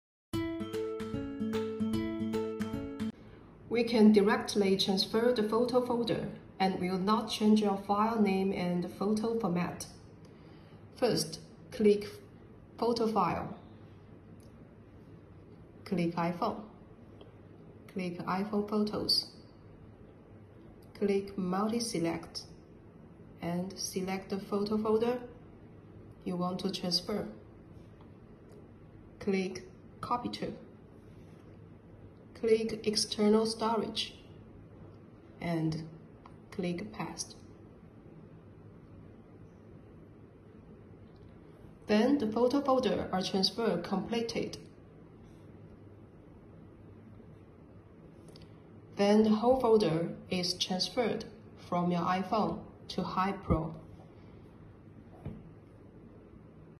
PL ZMPWLQ Photo Memory Stick-Transfer Photo Folder Function Operation Steps
How to Transfer Photo Folder from iPhone to PL Memory.